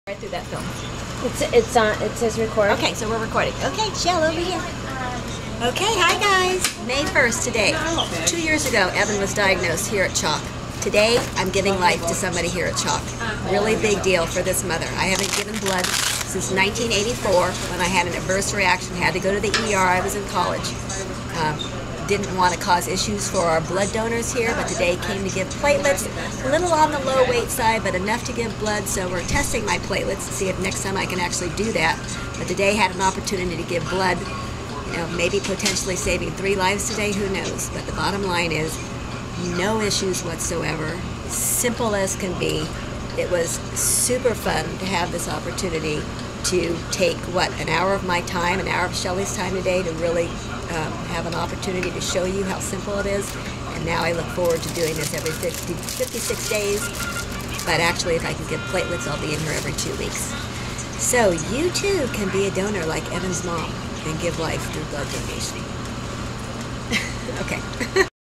Evan's Mom Donates Blood: BAHBAD.Org
After 25 years and a bad reaction to blood donation in college, Evan's Mom finally makes weight and is able to donate blood, potentially saving three children's lives, in the same hospital that Evan was diagnosed two years ago today.